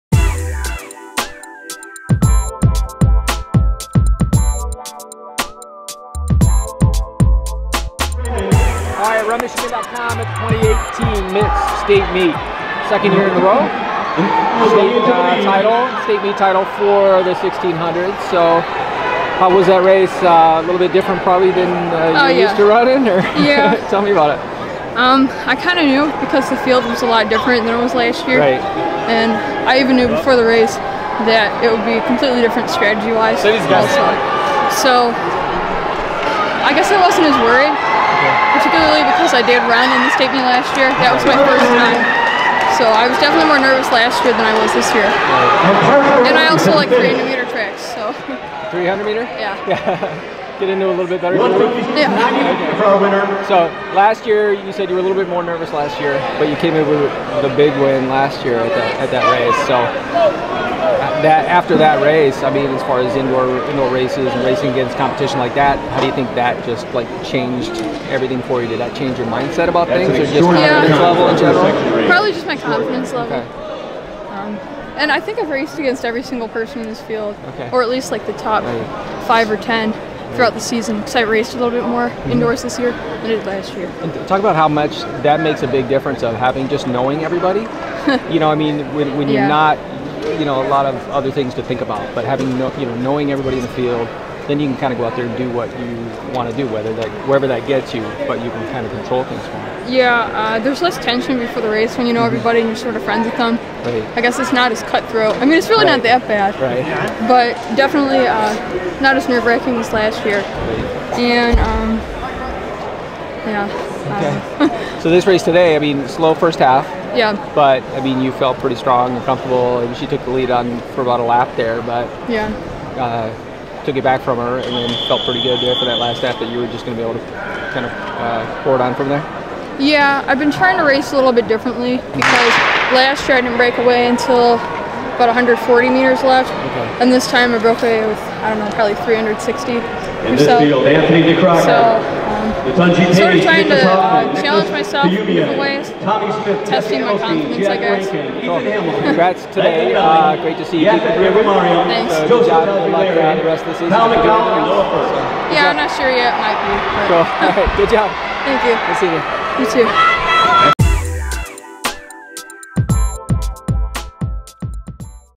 Interview: Karenna Duffey, 2018 MITS State Meet Girls' 1600 Champion
- RunMichigan talks with 2018 MITS State Meet Girls' 1600 Champion, Karenna Duffey of Breen Track Club, who took the state title with a finals time of 5:00.70.